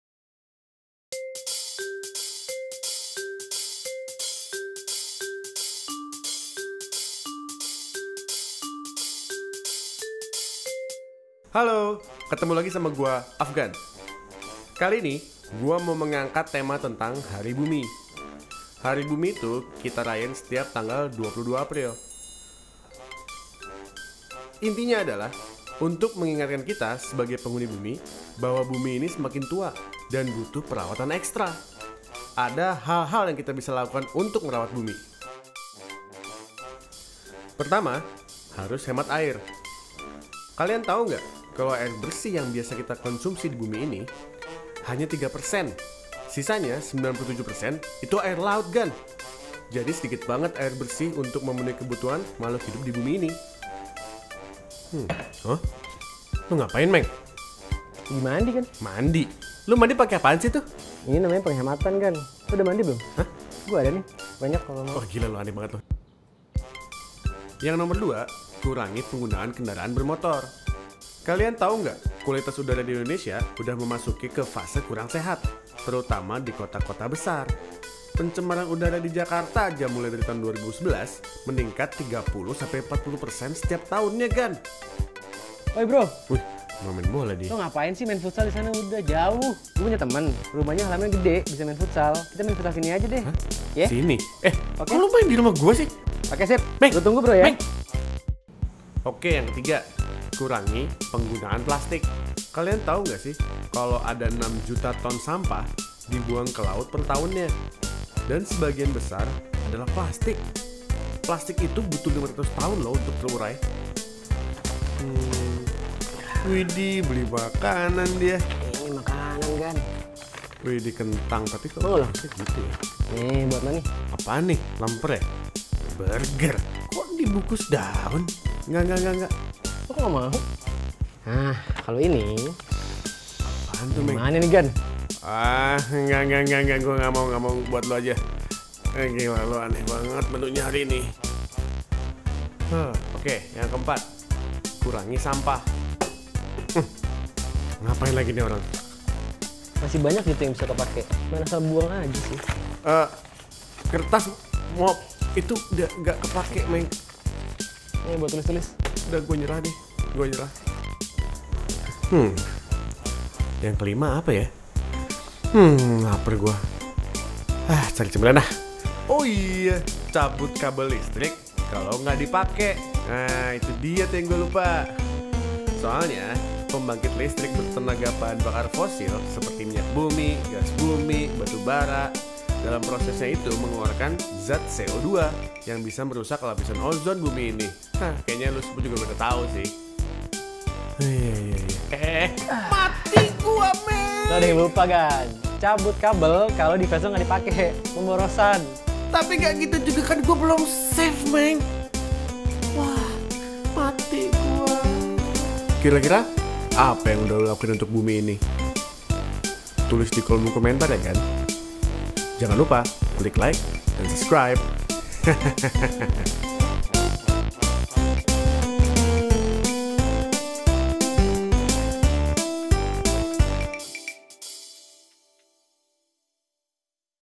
AFGAN & KOMENG Tips Merayakan Hari Bumi Ala KASKUSER
Halo Gan!

Selamat Hari Bumi ya! Kalau dipikir-pikir kita bisa hidup nyaman karena oksigen gratis ya dan kelihatannya air dan udara masih tersedia aja,

But, sadar ga sih, walaupun tersedia gratis, air dan oksigen juga sudah berkurang jauh kualitasnya.

Nah, 5 Tips ini akan membantu kamu menyehatkan Bumi

Produced By: OKANE Media